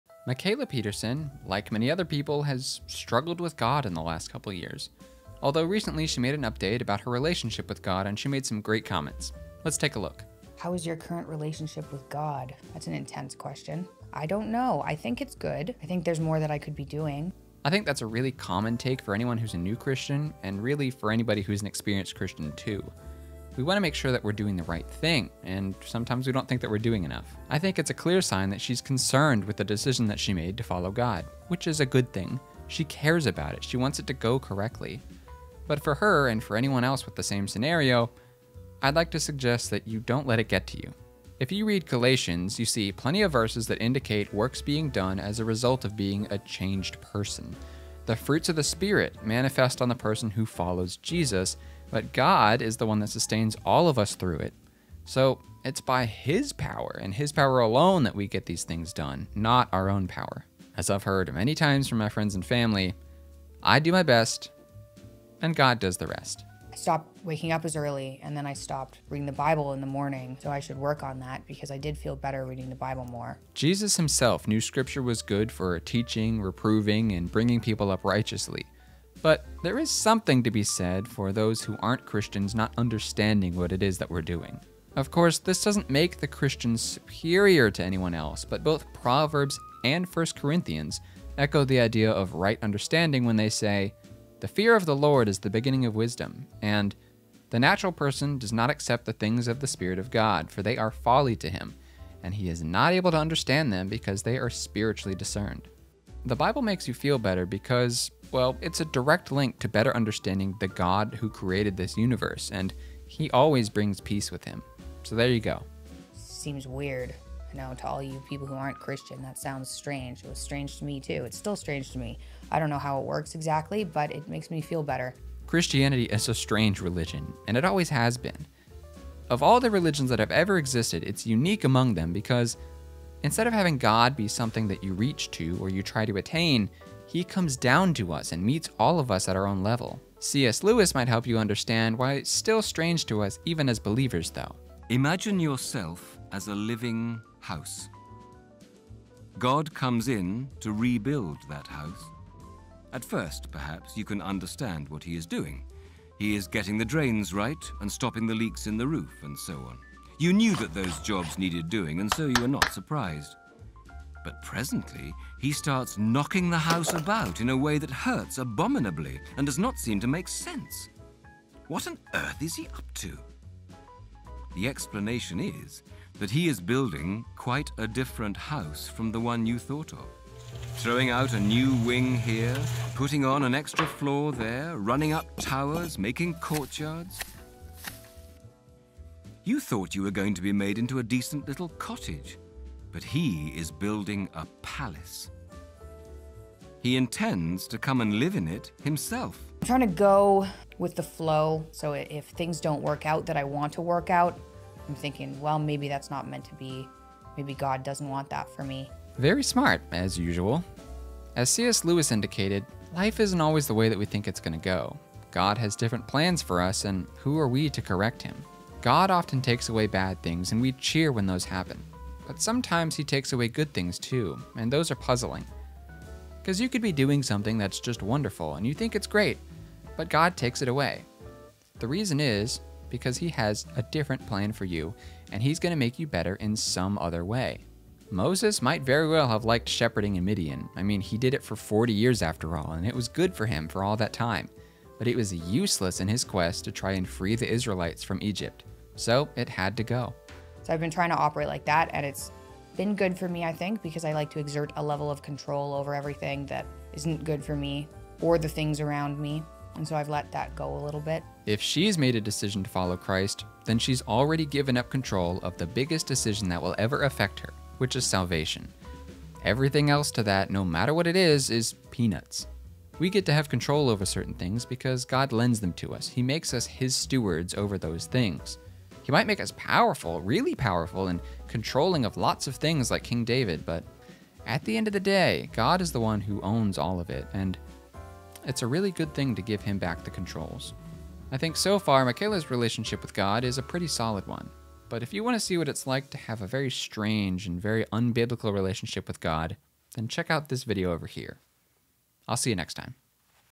Mikhaila Peterson's Relationship with God
Watching the Peterson family wrestle with God publicly has been astounding to experience. Take a look at Mikhaila's newest update on God.